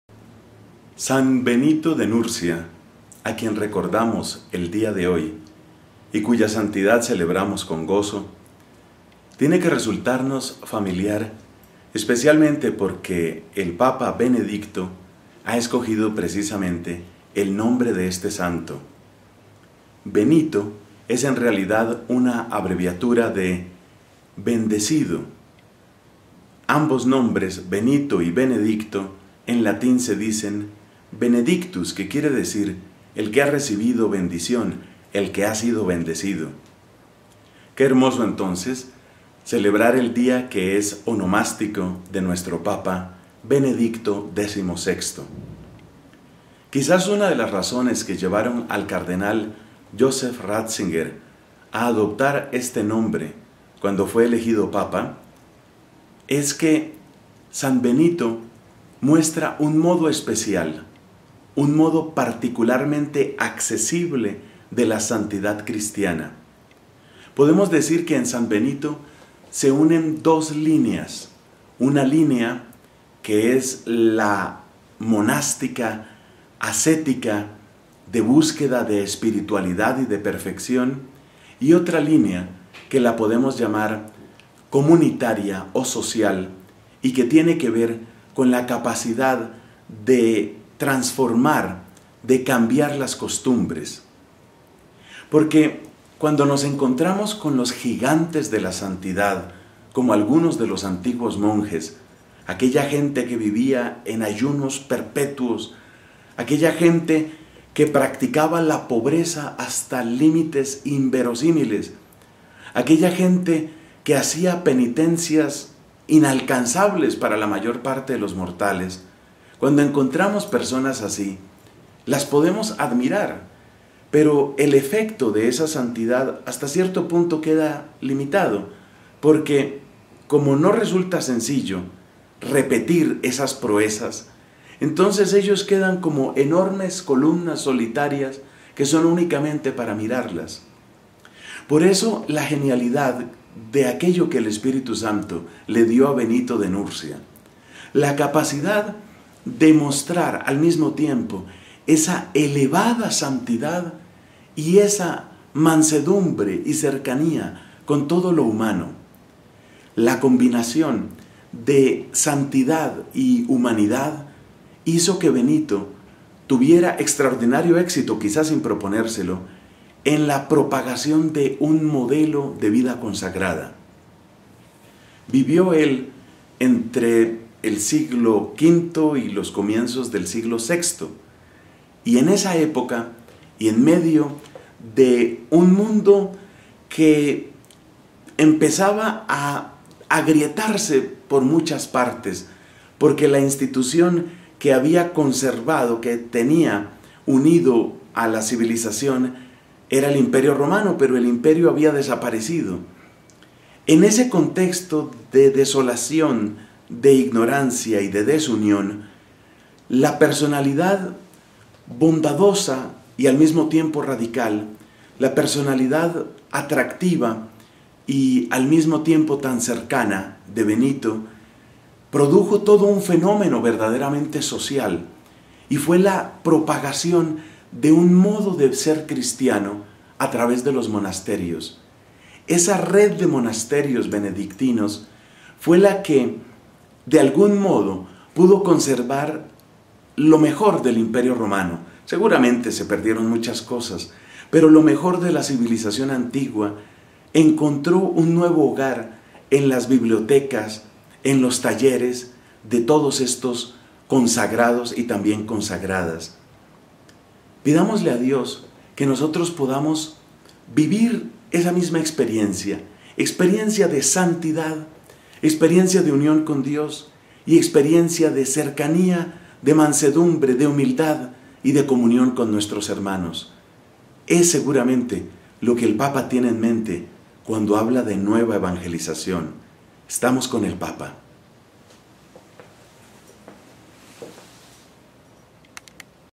LA GRACIA del Miercoles 11 de Julio de 2012
MEMORIA DE SAN BENITO DE NURSIA

Santidad y bondad se unen en San Benito de modo tal que su ejemplo cambió la faz de lo que hoy es Europa.